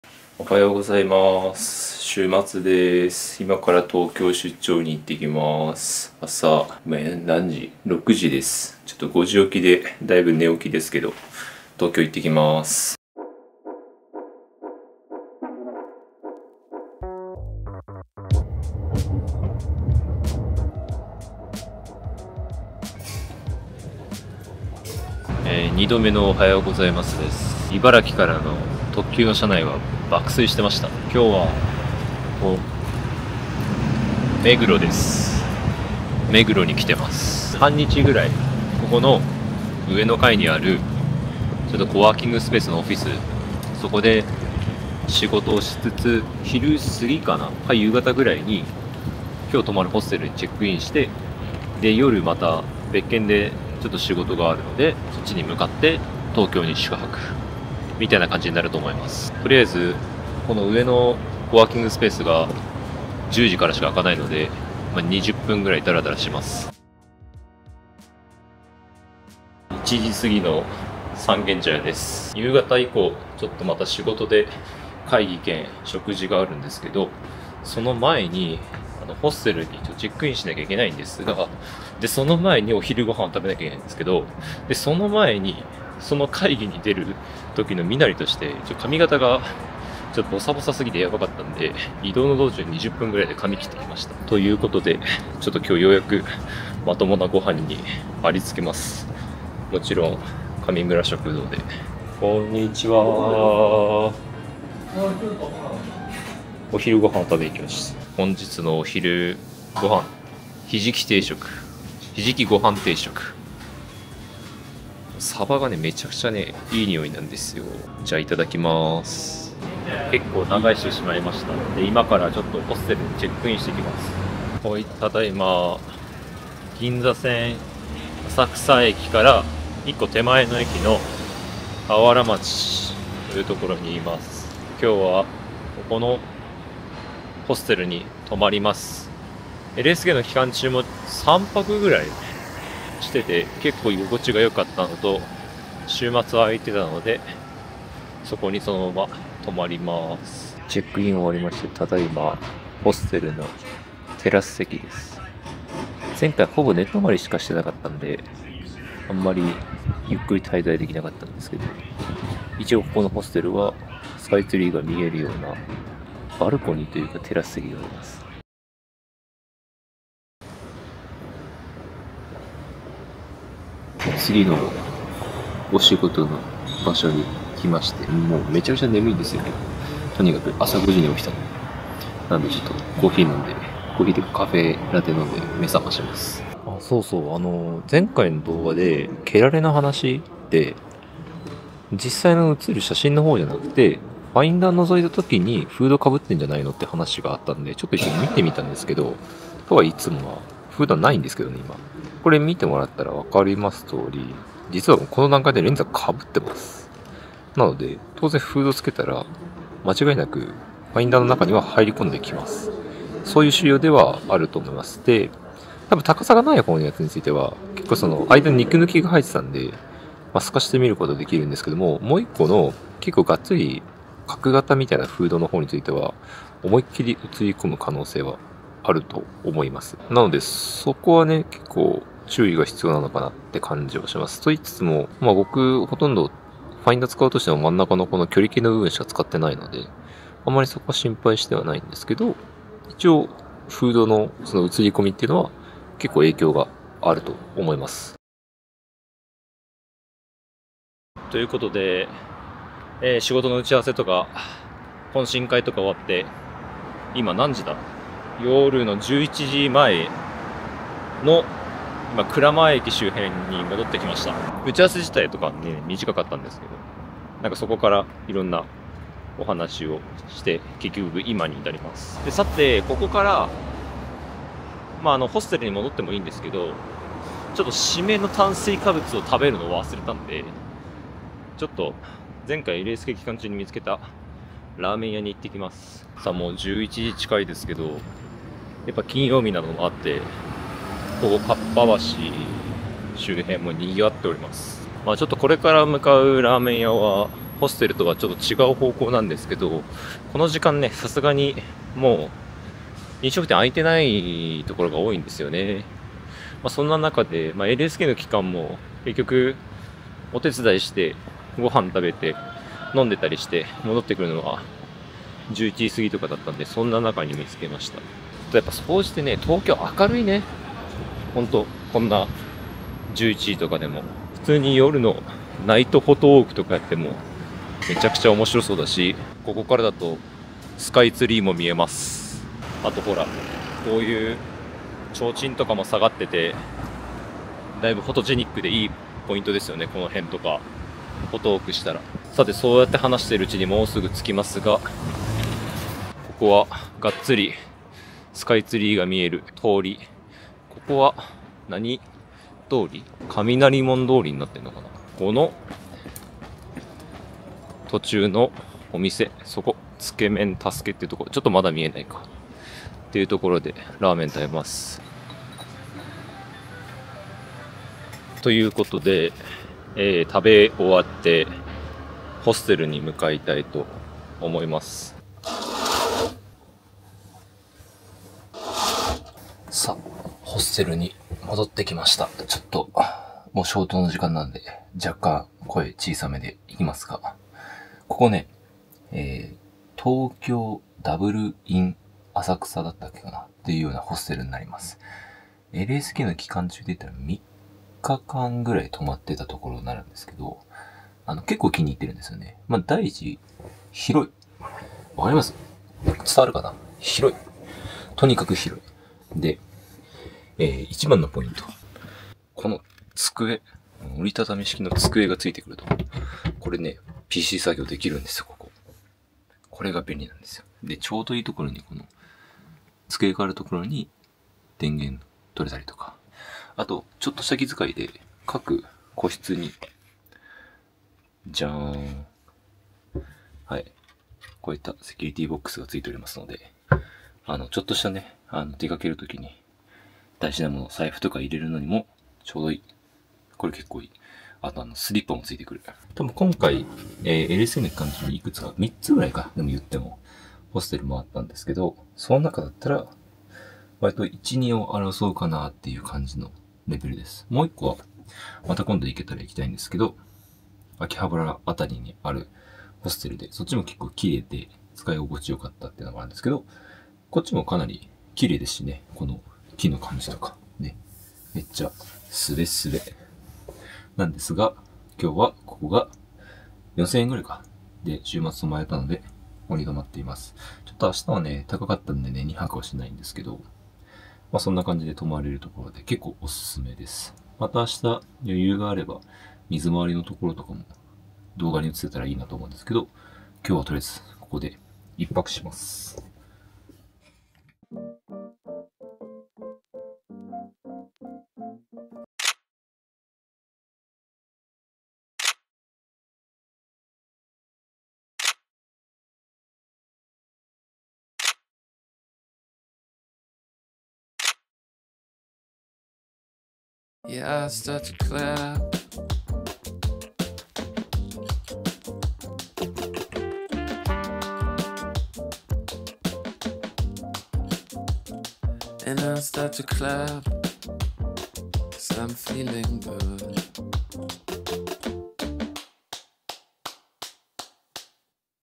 思いつきで20分1,800円で髪を切ったが想像より良かった。ー | Leica M11 monochrom
身だしなみは整えた方が良い。

Camera
- Leica M11 monochrom
- Leica Q2 monochrom
- Sony α7CR
- Sony ZV-E1

Action Cam.